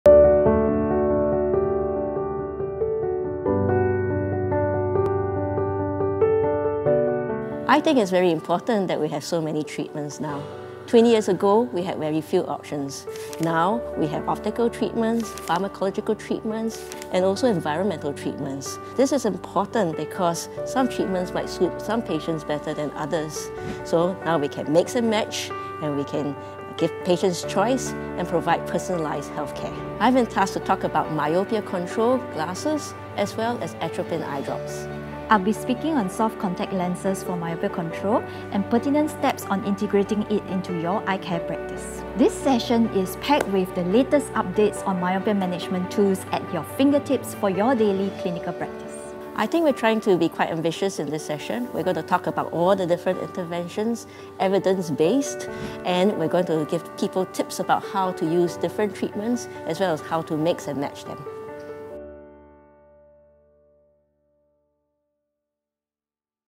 Asia-Pacific Myopia Management Symposium: Overview of Myopia Treatment Options
Join Media MICE as we examine one of the most critical issues in ophthalmology in the Asia-Pacific region; Myopia. The condition has reached pandemic levels of prevalence and the community is increasingly focused on finding new forms of treatment to manage the disease. Featuring Assoc. Prof.Audrey Chia, Dr. Foo Li Lian, Prof. Pauline Cho, and Prof. Saw Seang Mei the symposium examines how optical, pharmaceutical, environmental treatments, and others can help find the right match for patients. Our thanks to our friends at CooperVision & SNEC for putting this symposium together.